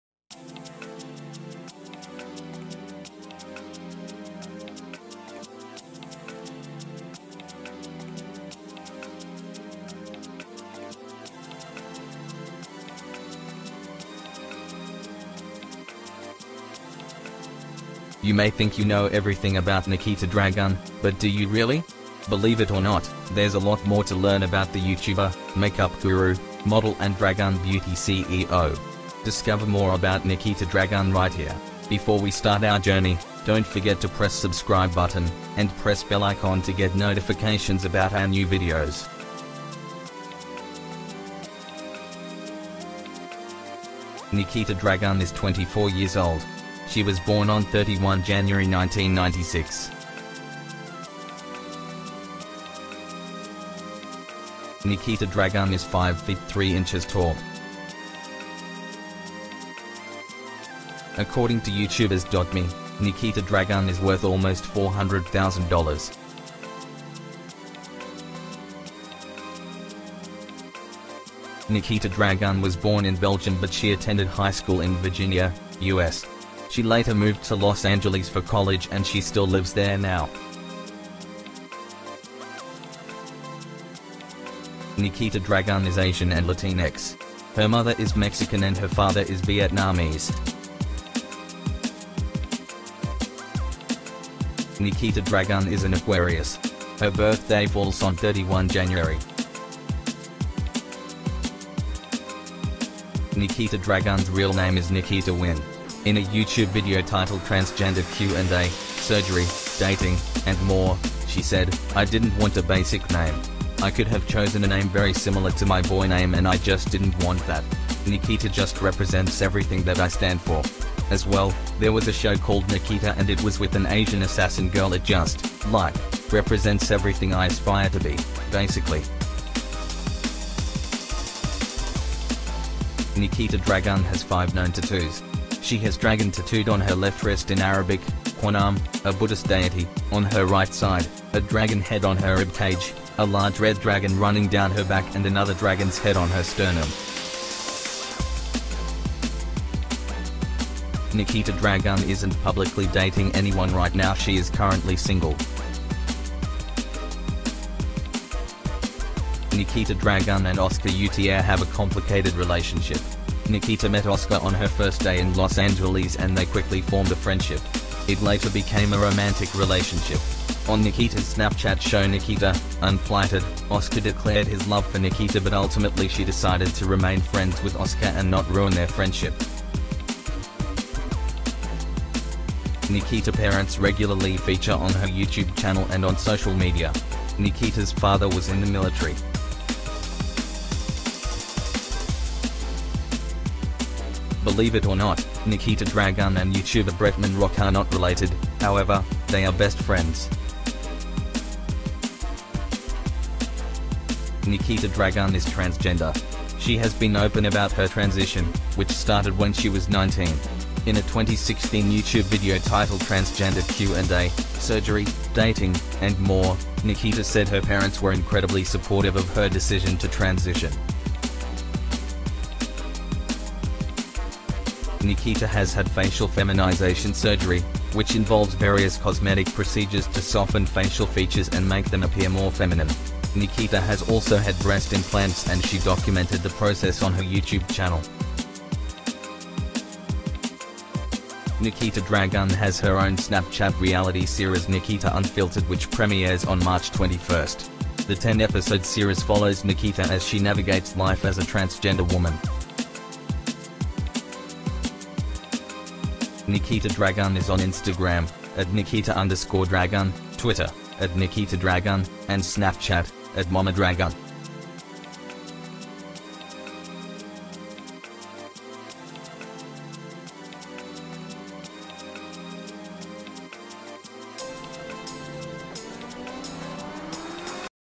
Nikita Dragun (Dragun Beauty) 17 Facts About the YouTuber You Probably Never Knew/ Celebrity Reality
Find out Nikita Dragun's age, height and everything
else about the YouTuber right here.

You may think you know everything about
but do you really? Believe it or not, there's a lot
more to learn about the YouTuber, makeup guru, model
and DragunBeauty CEO. Discover more about Nikita
Dragun right here.